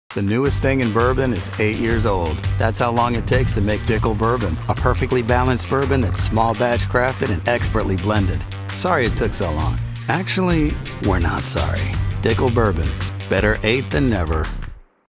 Dickel Bourbon
George Dickel has announced the newest release from the award-winning Cascade Hollow Distillery – Dickel Bourbon – a permanent offering that’s been aged 8 years.